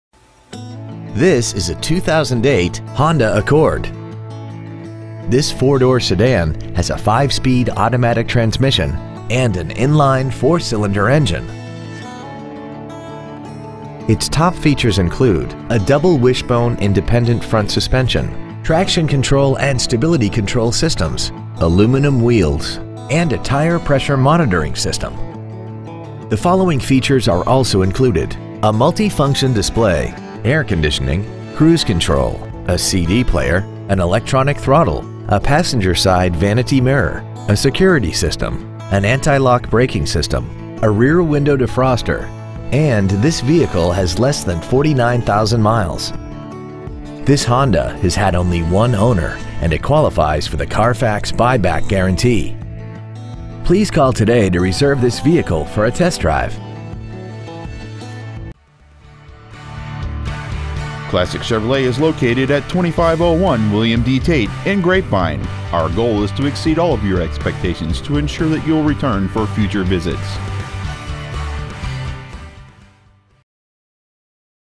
2008 Honda Accord Sdn Arlington TX
Year : 2008

 Make : Honda

 Model : Accord Sdn

 Engine : Gas I4 2.4L/144

 Trans . : Automatic

 Exterior : Alabaster Silver Metallic

 Miles : 48,194

 Interior : Gray

 Classic Chevrolet

 2501 William D Tate

 2008 Honda Accord Sdn Grapevine TX